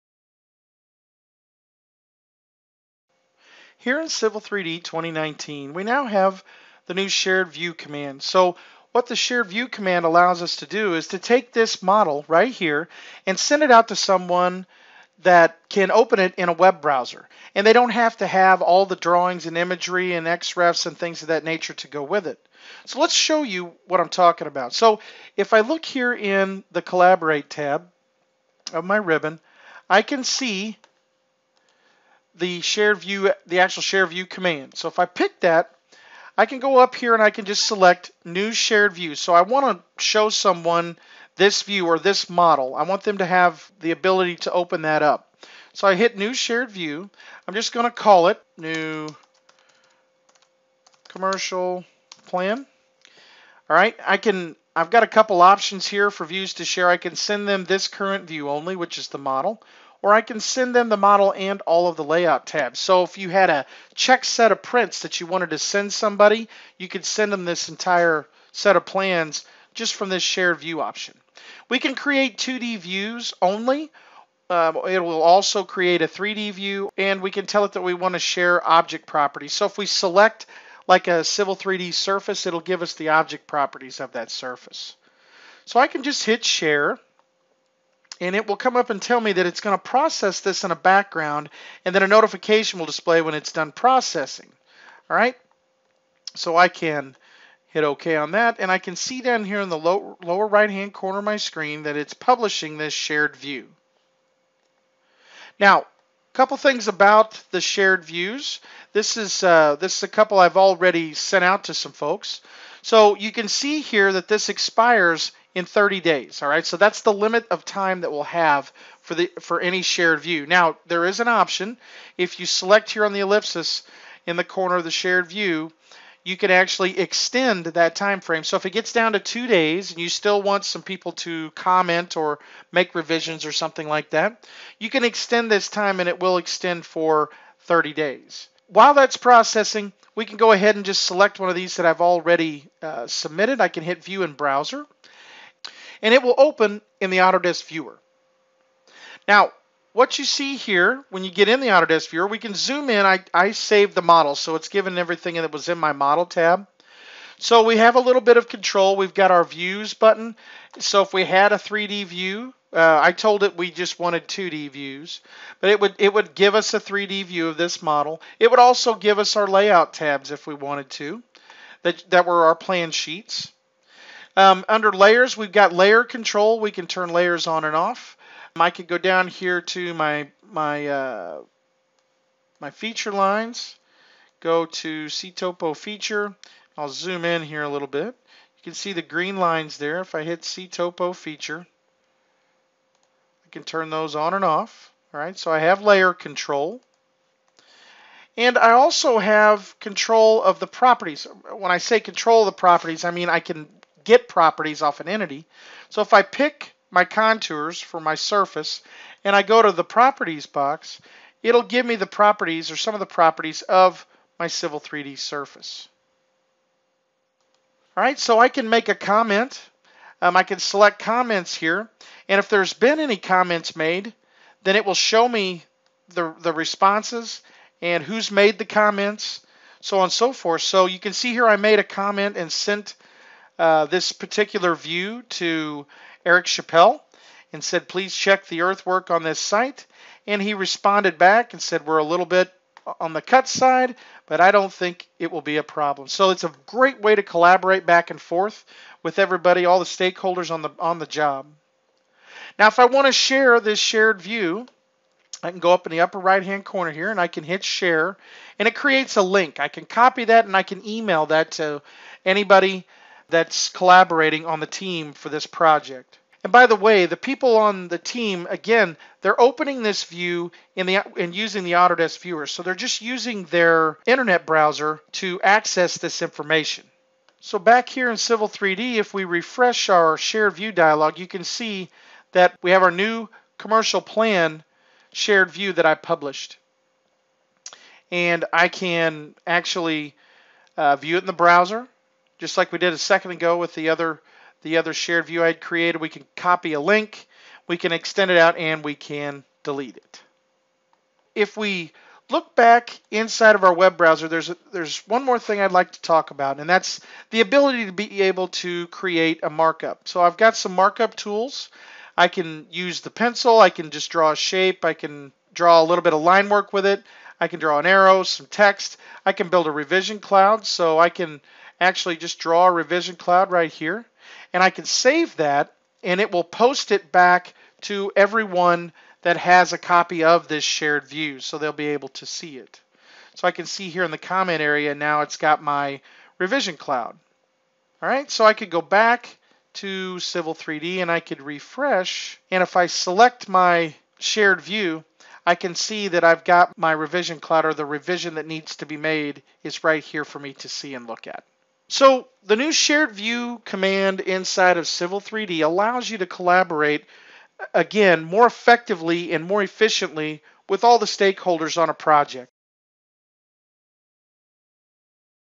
What's New in Civil 3D 2019 - Using the Shared View Feature
In this video we will go through how to use one of the new features in Civil 3D 2019. Using the Shared View Feature.